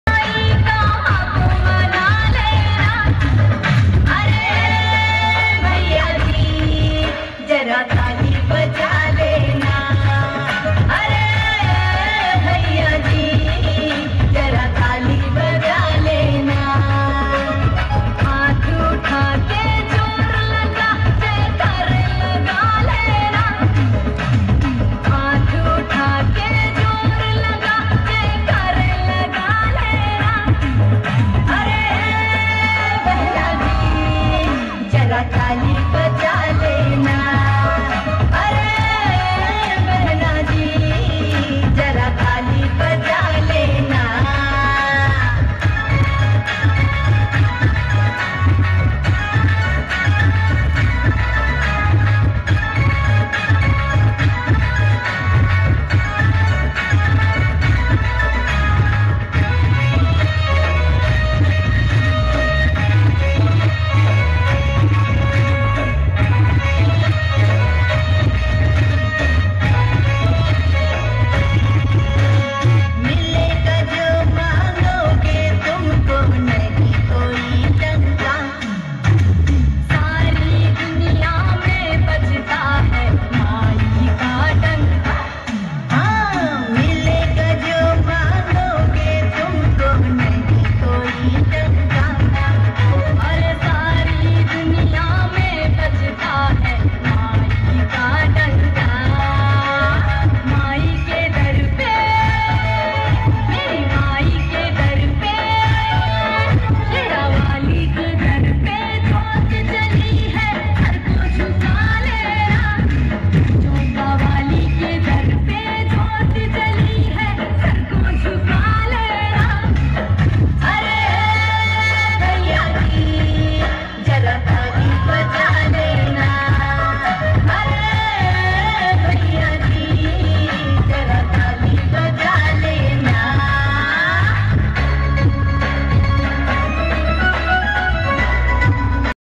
Tali Baja Lena Behind Stadium Pathakhera
Navaratri 2018,,,Day 7,, Behind Stadium A mix version of songs_,,A Day of Kali Ji + Gorakhnath Yuddh Act ,,,,,it was fantastic experience for each n all